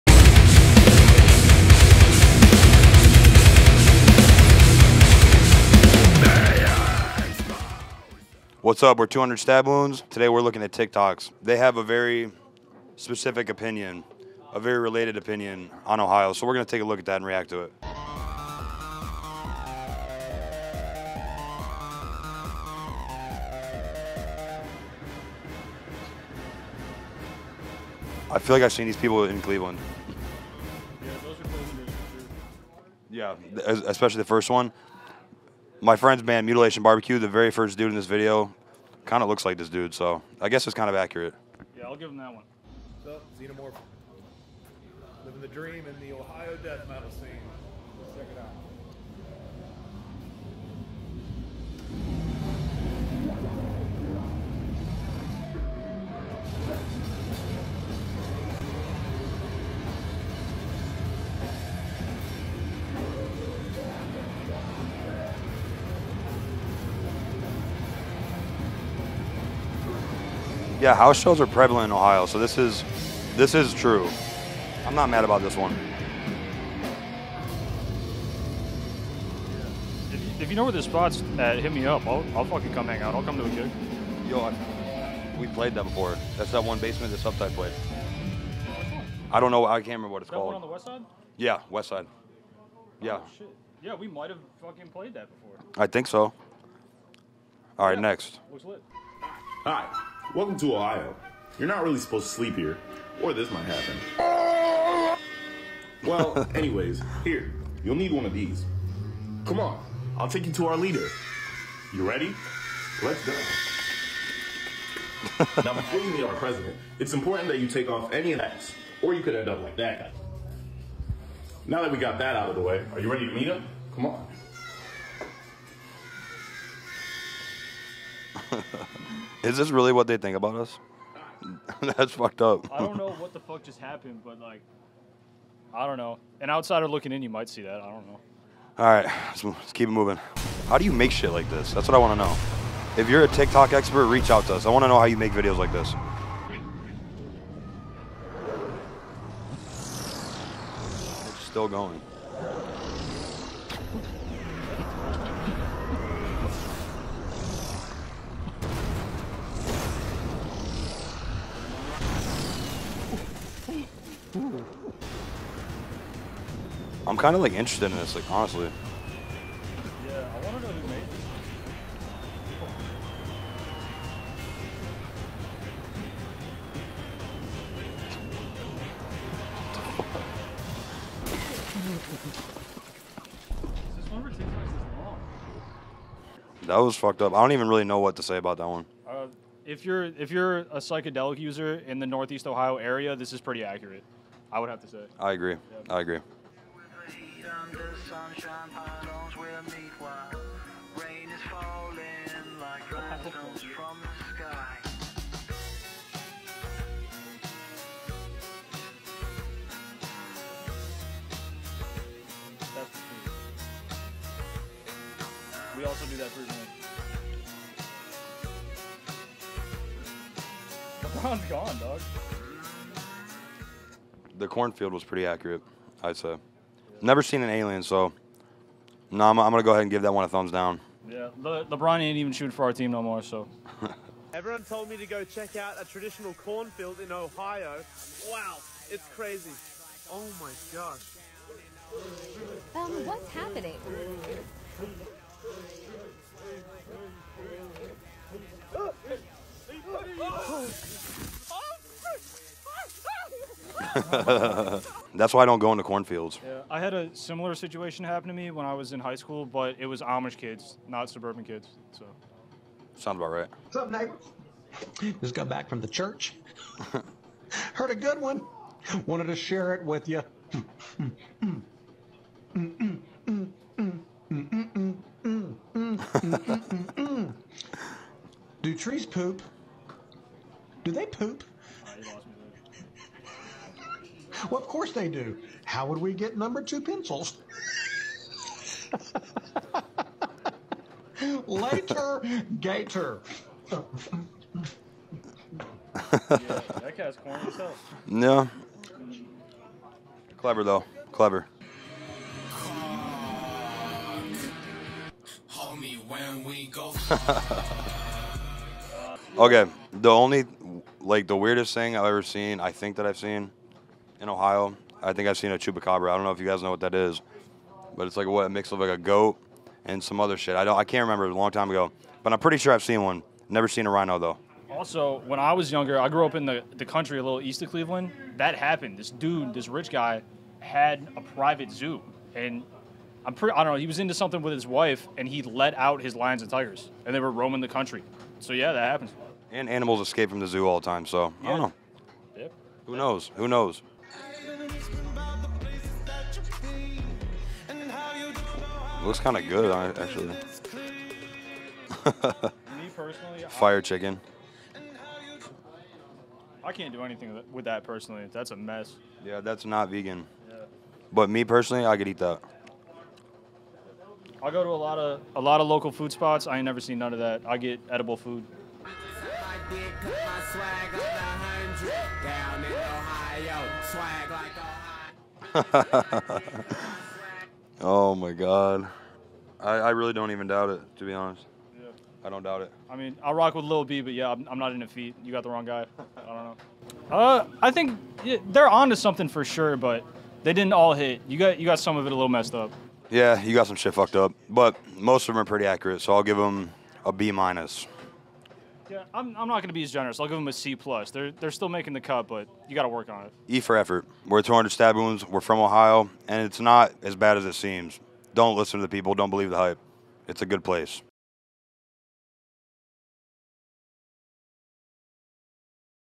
200 STAB WOUNDS Reacts To TikTok Videos About Ohio
Steve and Lance of 200 STAB WOUNDS sit down with Metalsucks to get to the bottom of the TikTok trend about living in Ohio. As a band from the 17th state, we grab their reaction to what people think of their home.